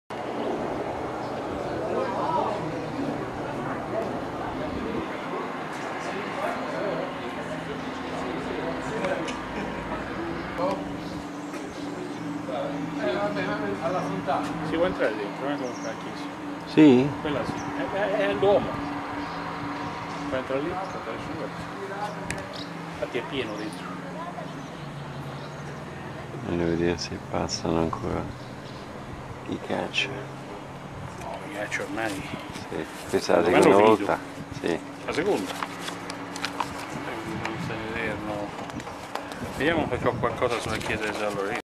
Viterbo, piazza S. Lorenzo, Cattedrale
La piazza di particolare bellezza presenta anche una elegante costruzione civile del XIII sec. [casa di Valentino della Pagnotta]